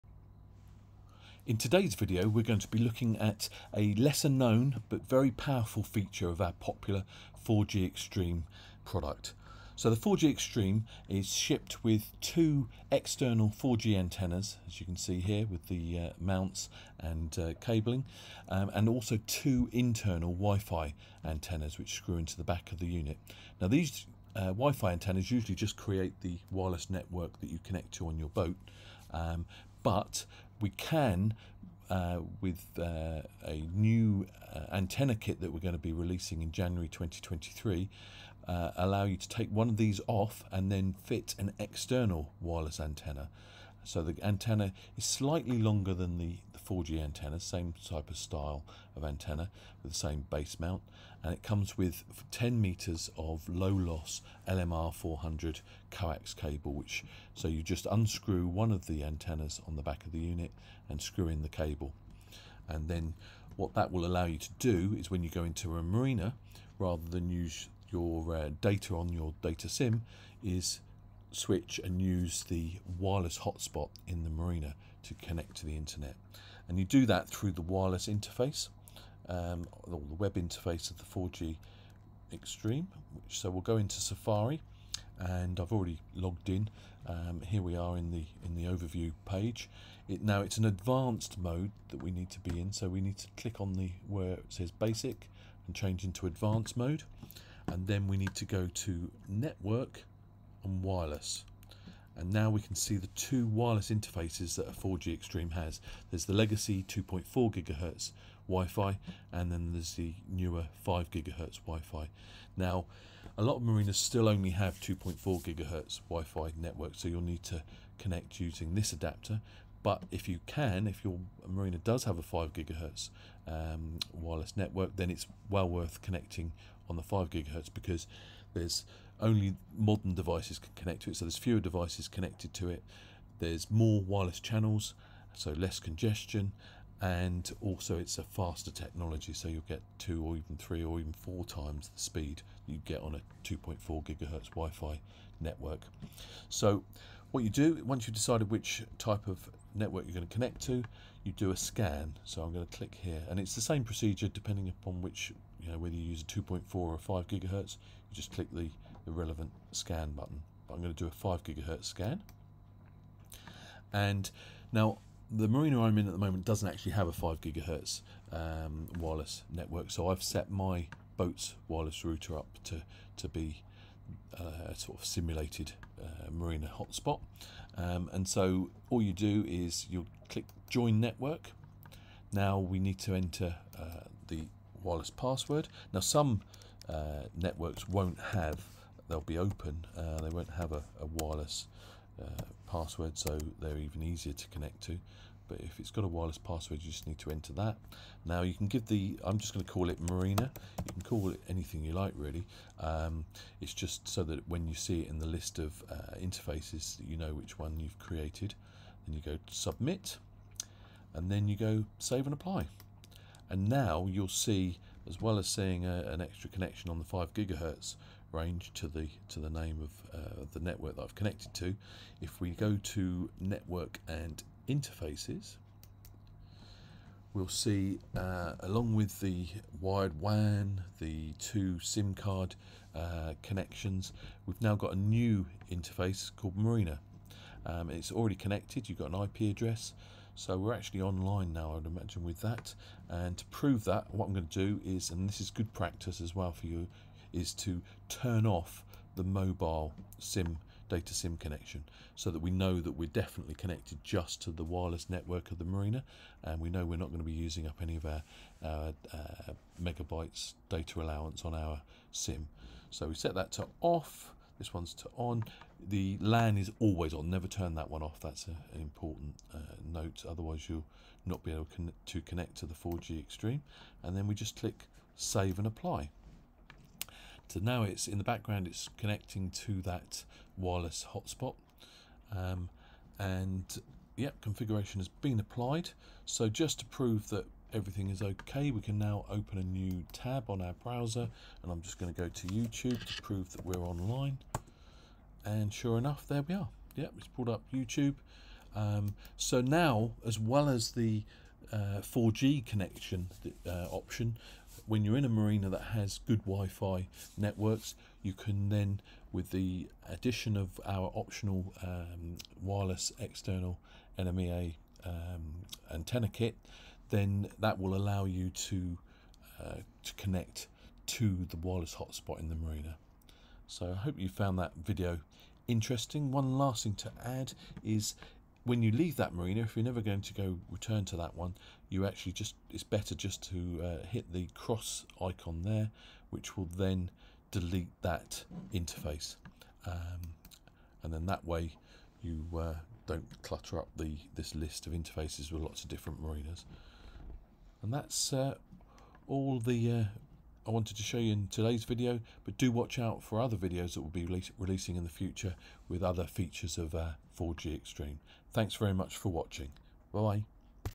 Connecting a 4GXtream to a wireless hotspot
This video explains how to connect the 4G Xtream from Digital Yacht to an external Wi-Fi hotspot (marina, bar, restaurant, etc.) instead of using the 4G internet connectivity.